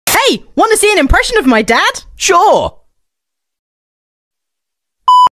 Wanna See An Impression of My Dad? // Cassie and Gregory // FNaF RUIN DLC // My FNaF AU
IM SORRY I HEARD THIS AUDIO AND THOIGHT OF CASSIE 😭

YK??

Characters/Ocs Used:
Gregory
Cassie [DLC Girl]

Watermark Credits!
୧ ׅ𖥔 ۫ Ichiko Yuri ⋄ 𓍯 ☁️ on YouTube!

2nd Watermark credits:
‎@SomeScottish_Potato  on youtube!

3rd Watermark credits!
me 🤩

4th Watermark Credits!
‎@justxstxrry on youtube!

Random Tags (Ignore)